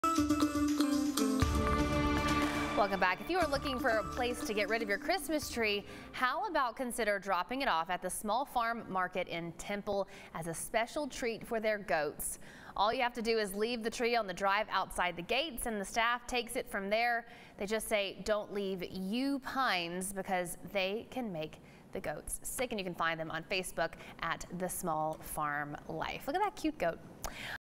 Temple farm asks for Christmas trees
Need somewhere to depose of your Christmas Tree? Well this Central Texas farm would love to take it off your hands.